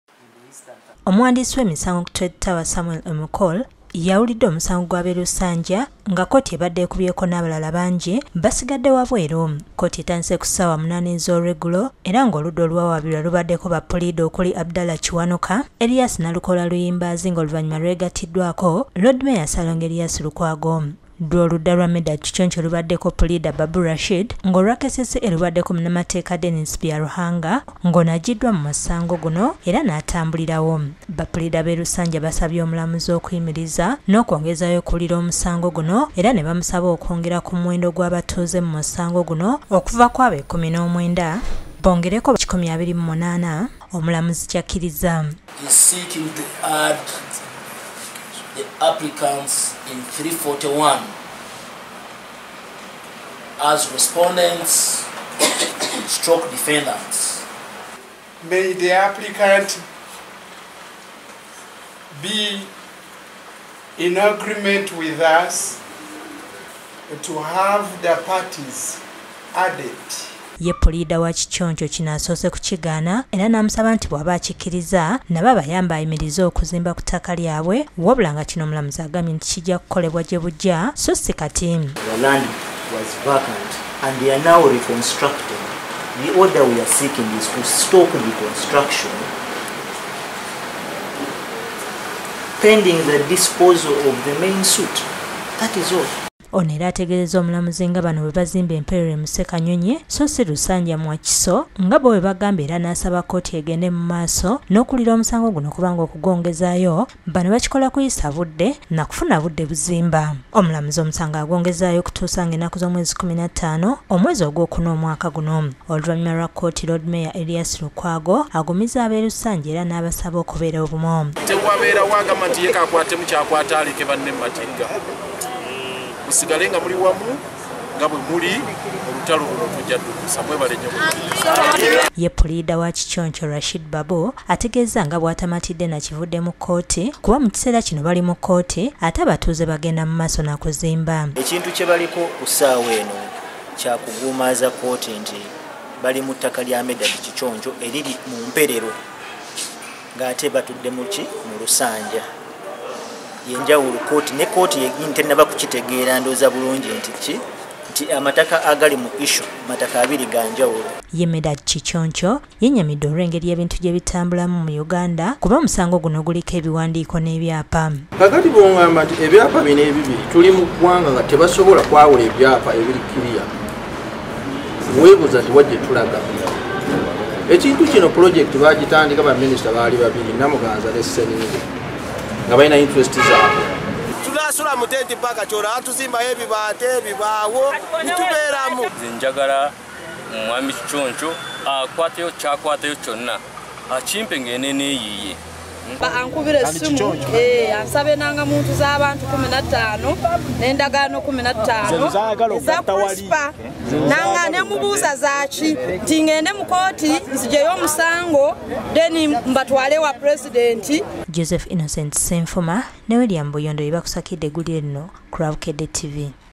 COURT HAS STARTED HEARING THE LUSANJA LAND EVICTION CASE
The court room was full to capacity and other people had to remain outside